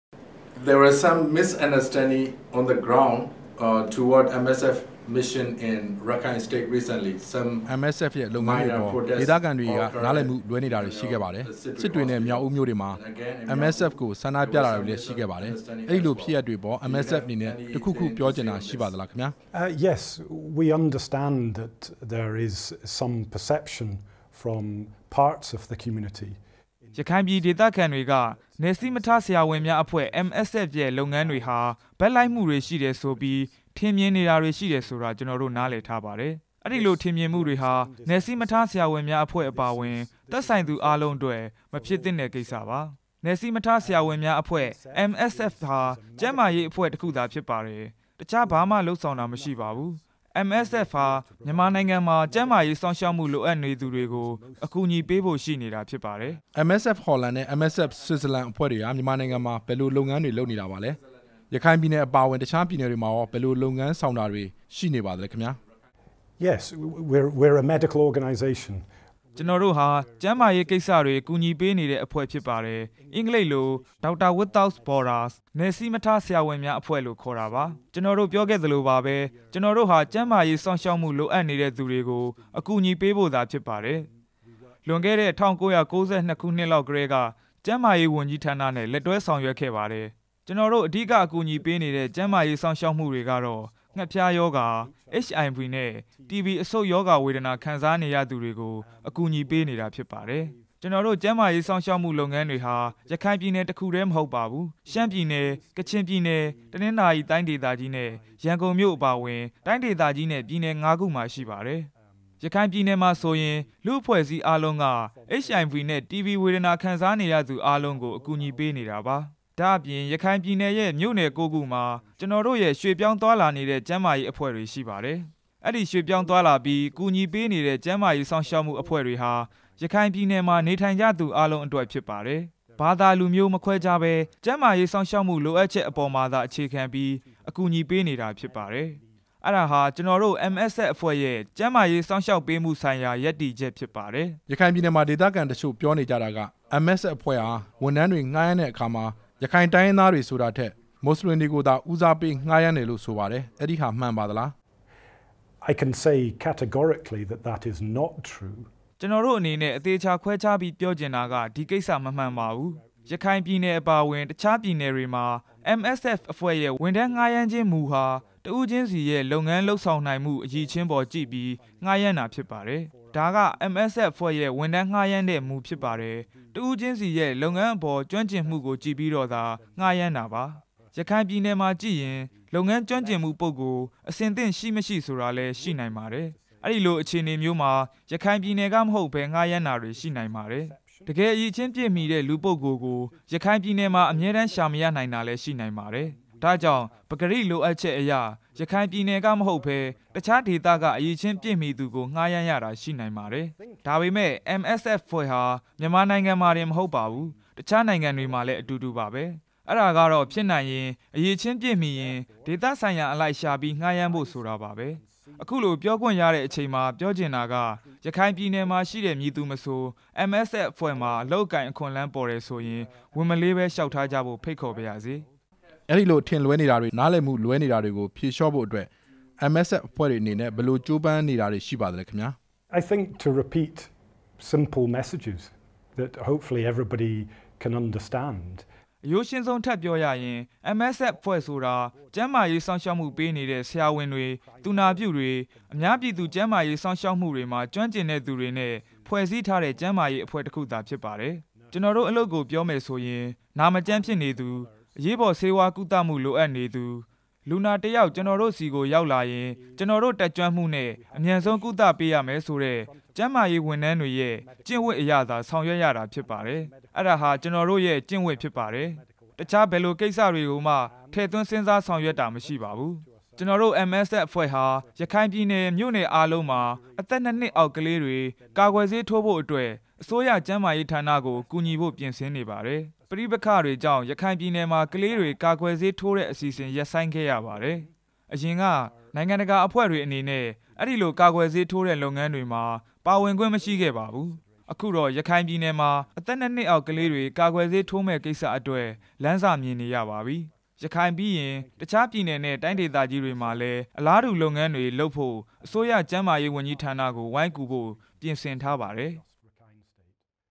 Duncan Bell, Chief of MSF (Switzerland) on Aid Bias Allegations in Rakhine
ရခိုင္ျပည္နယ္က က်န္းမာေရးေစာင့္ေရွာက္မႈ လုပ္ငန္းေတြမွာ အကူအညီေပးေနတဲ့ "နယ္စီးမထား ဆရာဝန္မ်ားအဖဲြ႔" MSF ဟာ ဘက္လုိက္မႈ ရိွေနတယ္ဆုိၿပီး ေဒသခံ ရခိုင္တုိင္းရင္းသားေတြက ဆႏၵျပတာဟာ MSF ရဲ႕လုပ္ငန္းေတြအေပၚ နားလည္မႈလဲြေနတာပဲ ျဖစ္တယ္လုိ႔ MSF အဖြဲ႔က ေျပာပါတယ္။ ဆြစ္ဇာလန္ MSF အဖဲြ႔ခြဲရဲ႕ ဌာေနကိုယ္စားလွယ္ Duncan Bell ကို RFA ဝိုင္းေတာ္သား ကိုဝင္းႏုိင္က ေတြ႔ဆံုေမးျမန္းထား ပါတယ္။